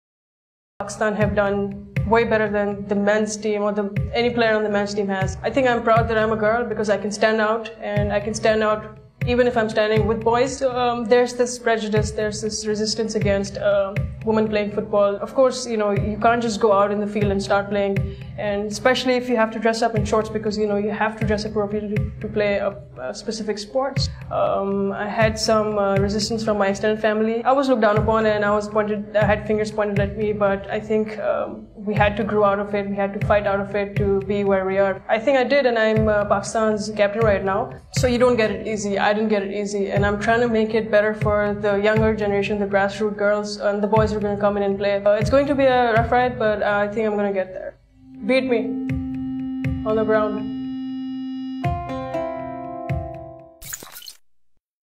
Hajra Khan BeatMe Pakistan Women Football Team Captain | UN Women Pakistan
Pakistani girls like Hajra Khan who is the captain of Pakistan Women Football Team, have broken stereotypes in the field of sports going against all the odds. Many can draw inspiration from the likes of Hajra Khan, Noorena Shams and Maria Toorpakai. Recently UN Women Pakistan’ started ‘BeatMe’ campaign that brings an unconventional twist to advocacy on women’s rights. In a compelling paradox, the campaign showcases a woman inviting a man to beat her - but at things she is good at. The campaign, aims to inspire women to reaffirm that they are stronger than they are made to believe, and aims to shatter the perception that a woman is weak, taking her from someone who is ‘beatable’ to being ‘unbeatable’.